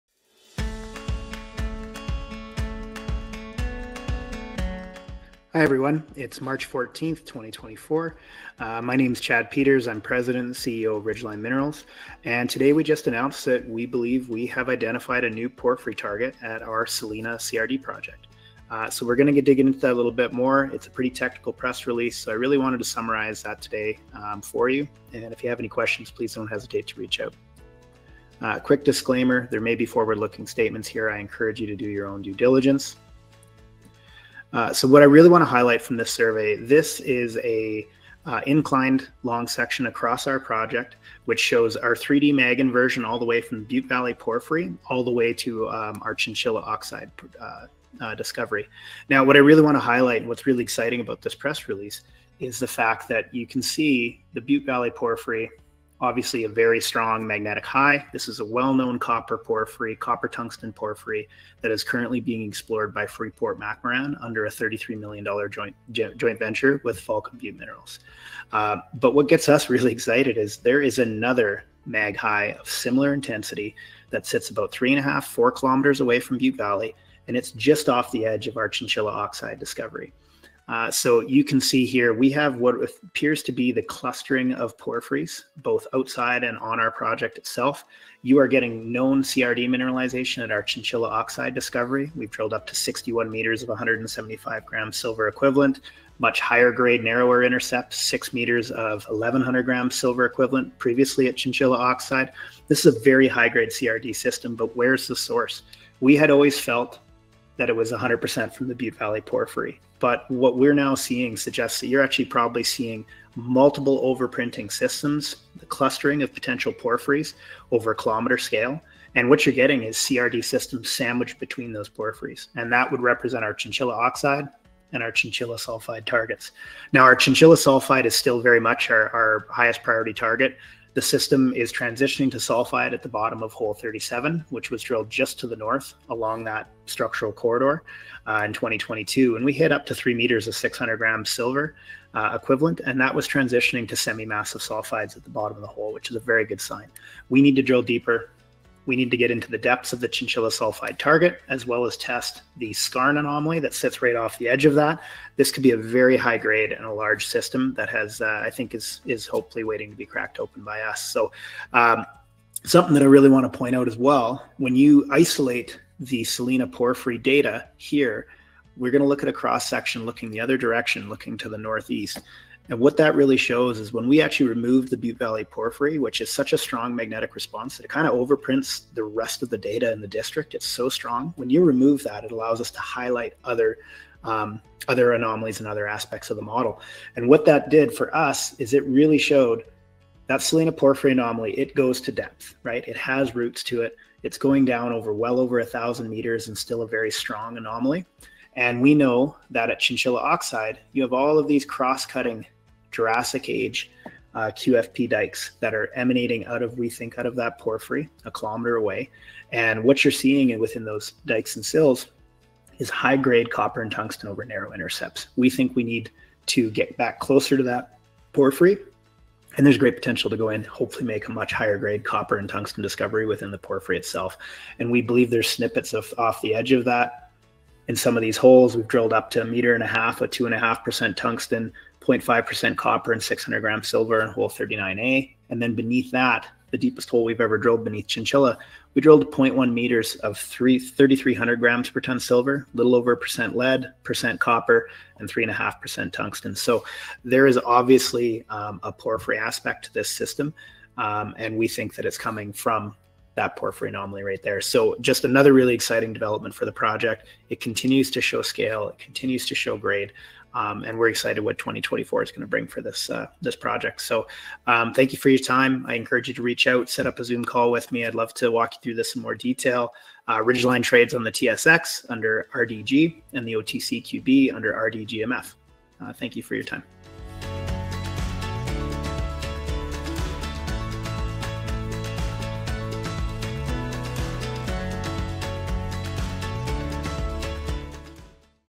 Ridgeline Identifies a New Porphyry Target at the Selena CRD Project in Nevada
Chad Peters, CEO, summarizes Ridgeline’s March 14th, 2024 press release highlighting a new porphyry target at the Selena project that was identified using high resolution drone magnetics.

Ridgeline Minerals is a Nevada-focused precious and base metal explorer with a portfolio of five highly prospective exploration projects in Nevada and a partnership with Nevada Gold Mines.